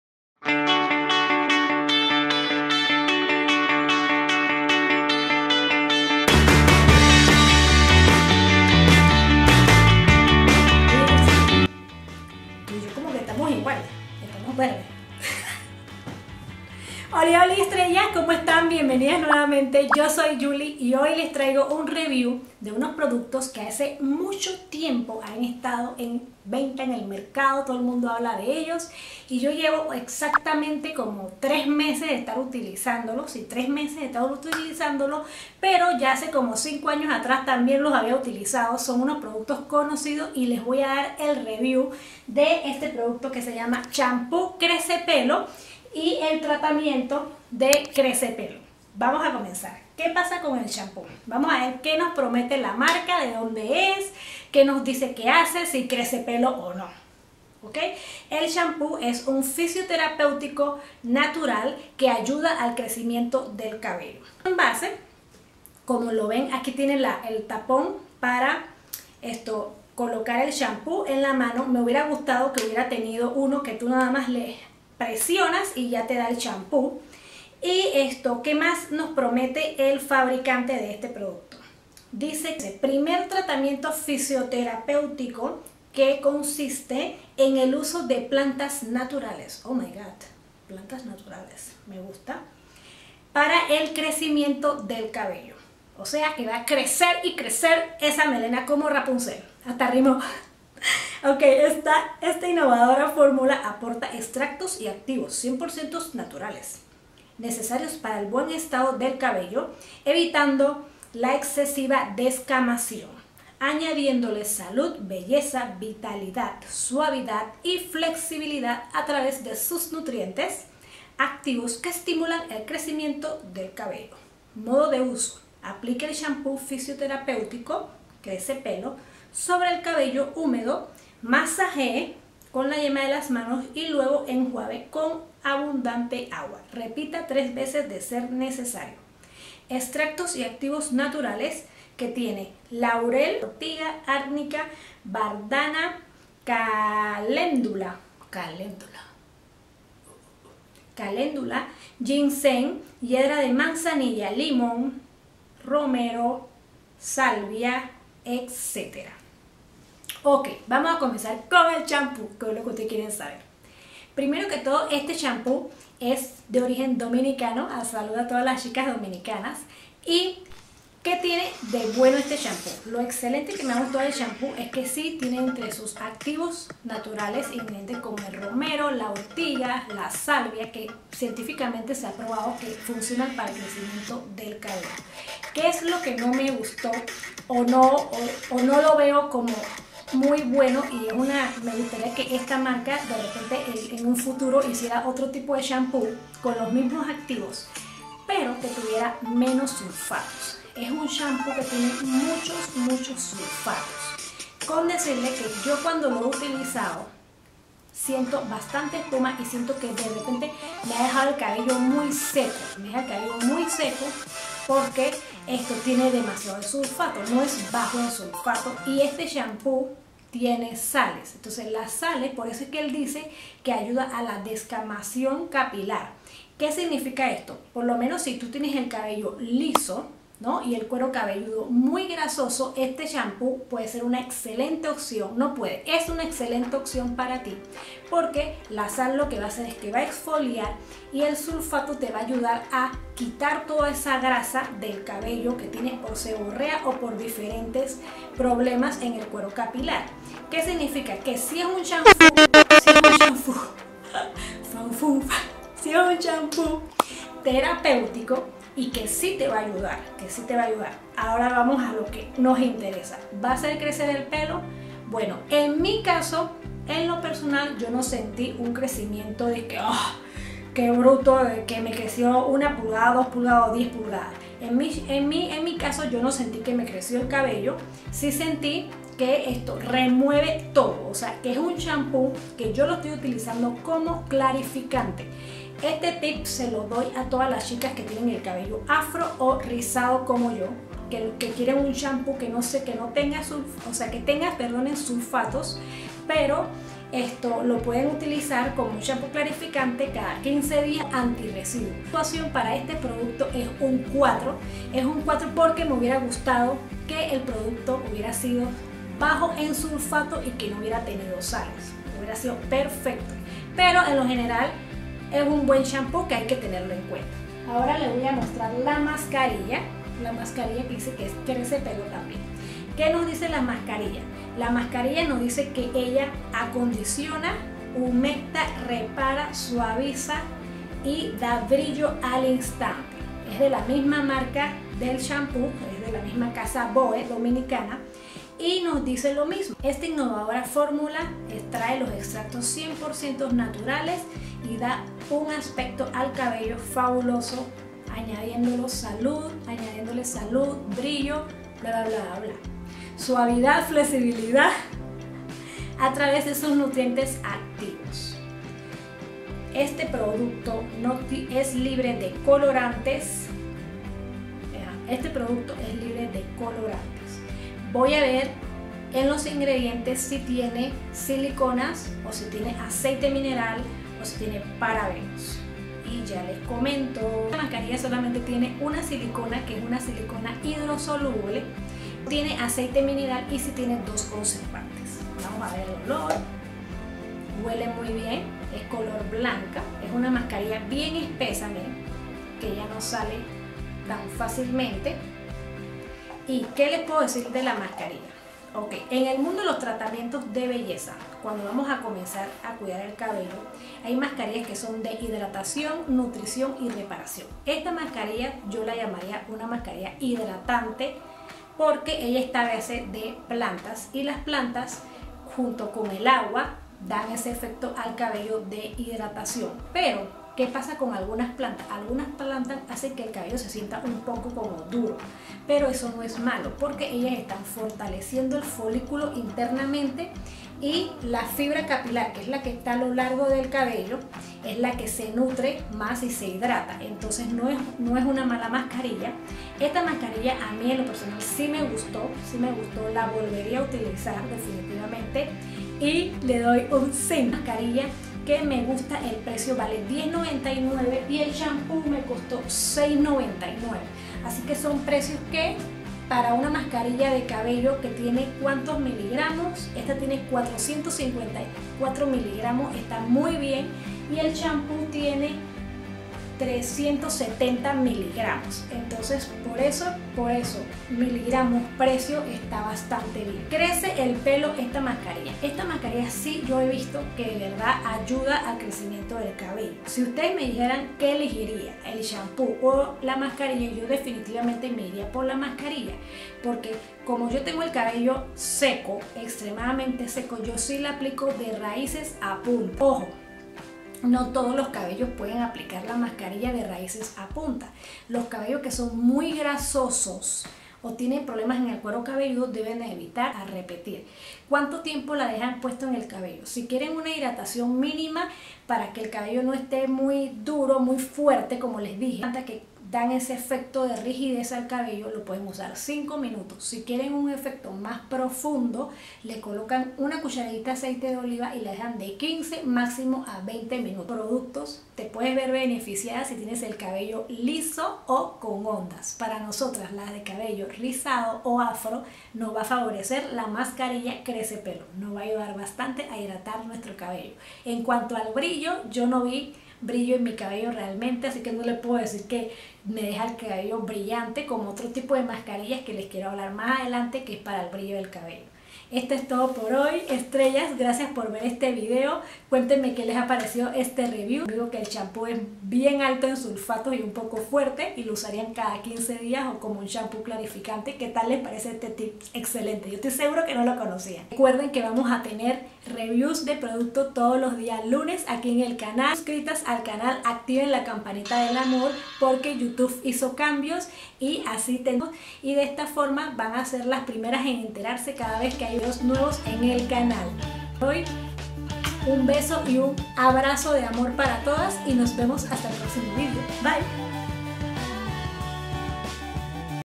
Review de Crece Pelo Tratamiento y Shampoo. Crece rápido tu pelo.
Crece Pelo Fitoterapeutico natural. Crece Pelo Natural Phitoterapeutic Treatment Help Capilary Growth.
Estos productos para el cabello prometen ayudar al crecimiento del cabello natural o cualquier tipo de cabello.
Hoy te comparto mi opinión personal, dándote tips para que le saques el máximo provecho.
Aquí te dejo el link para que le des clik y visites el blog donde te dejo fotografía e información necesaria.

Espero que te haya ayudado, y si es así, no olvides darle unos deditos arriba, y compartir con tu mejor amiga, para que también comience a cuidarse su melena y le crezca mucho, mucho en este nuevo año.